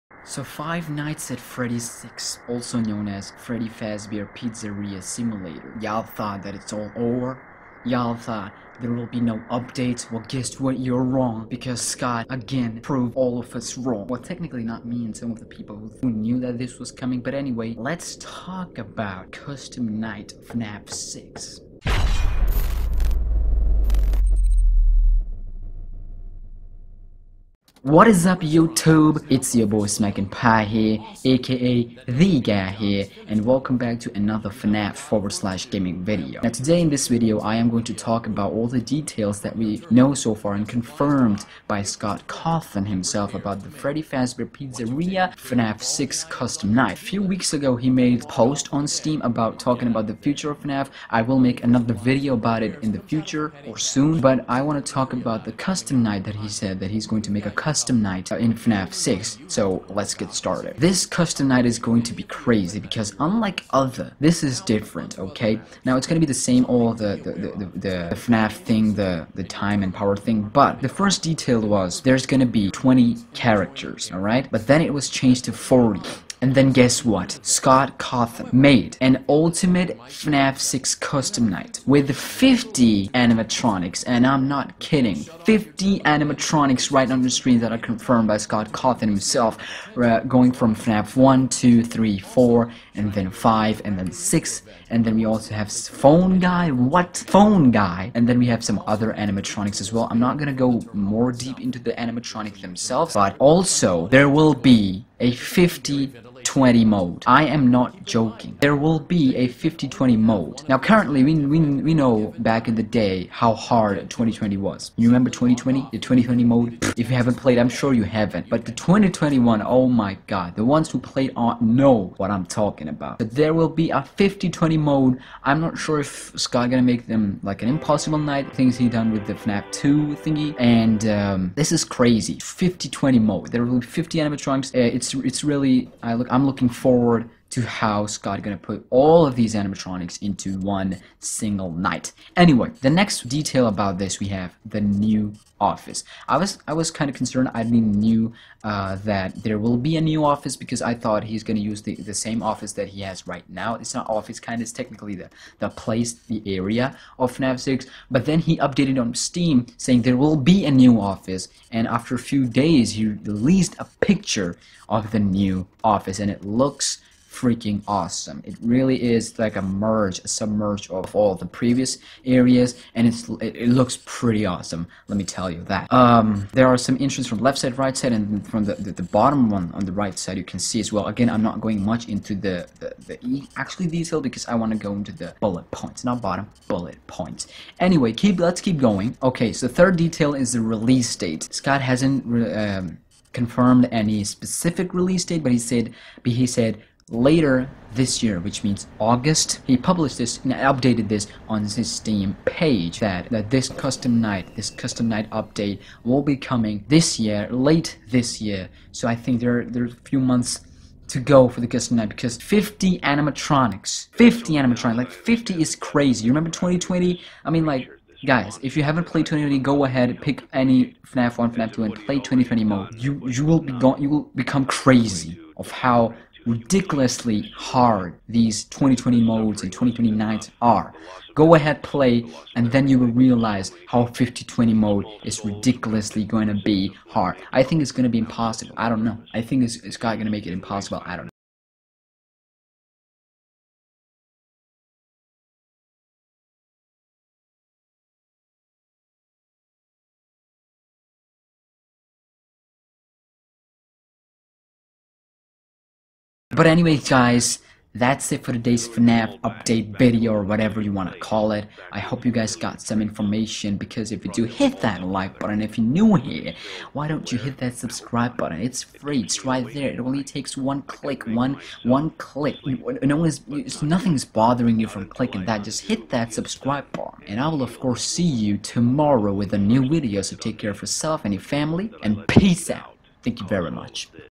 FNAF 6 Ultiamte Custom Night 50/20 All Details (Release Date, New Office, Progress Bar..)
Today in this video we're going to talk about FNAF 6 Ultimate Custom Night All Details we know so far about FNAF 6 Custom Night including it's release date, 50/20 mode, new office and more.

NEW MOVEMENT It'sEveryDay/NightBro-Sisz

But anyways guys, i'll see you soon...

SMACK
SMACKNPIE